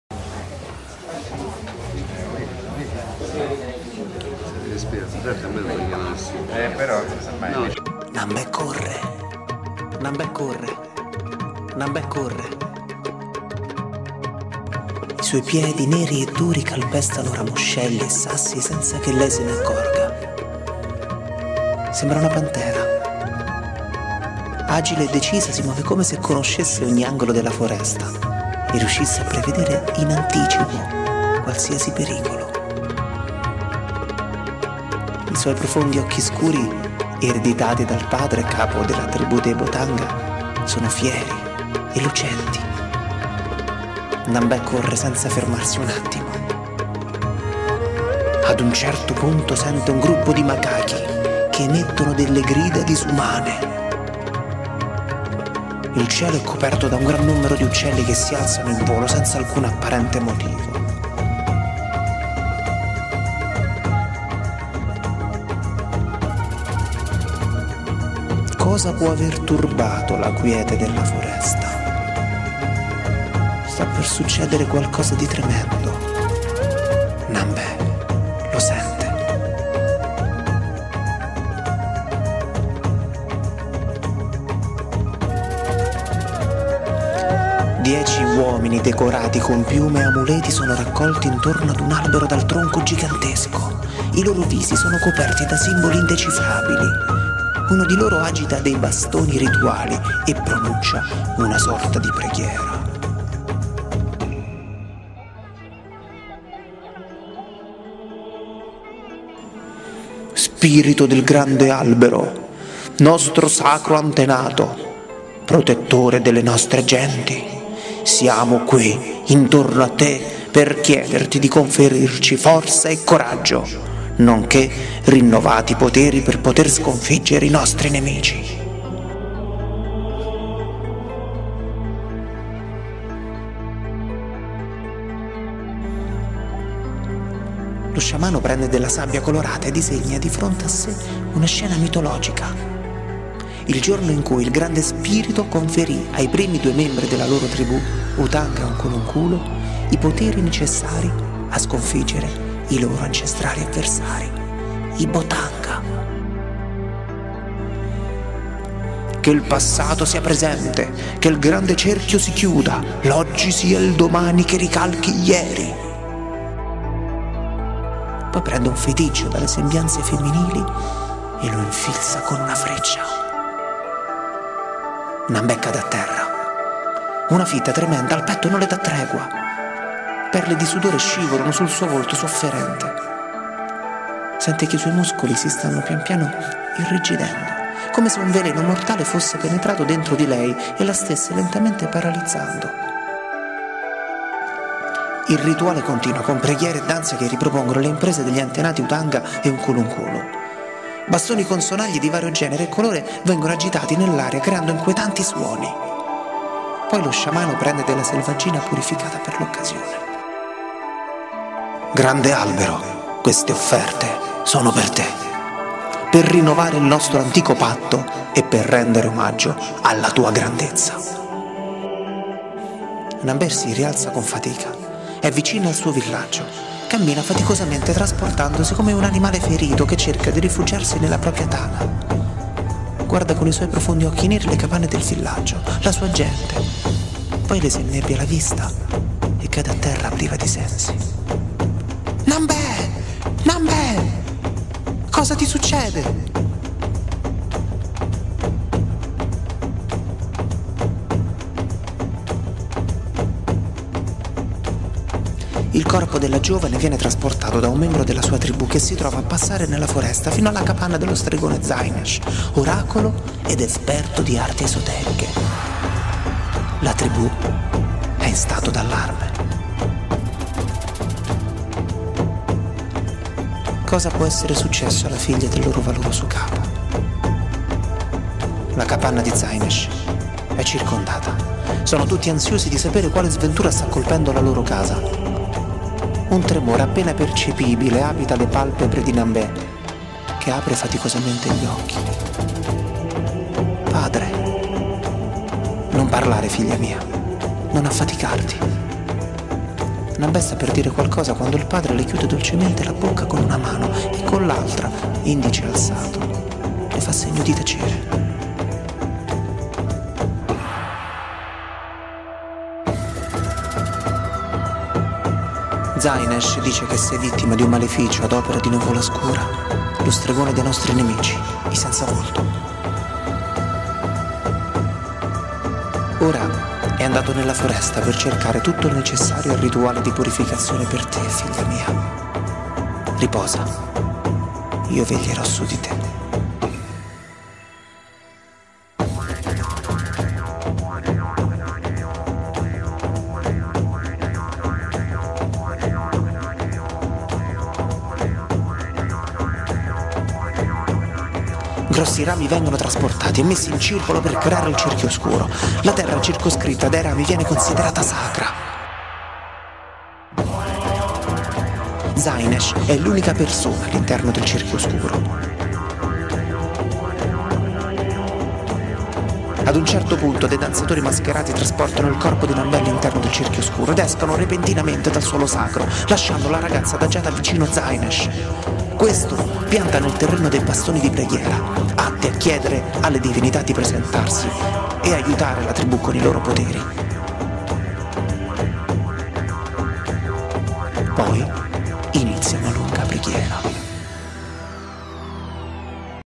Voodo di Emiliano Reali (prima parte)
Sul testo di Emiliano Reali, letto dall'attore Carlo Marchetti, danzano Alessandro Moroni, Silvia Spinogatti e Monia Felli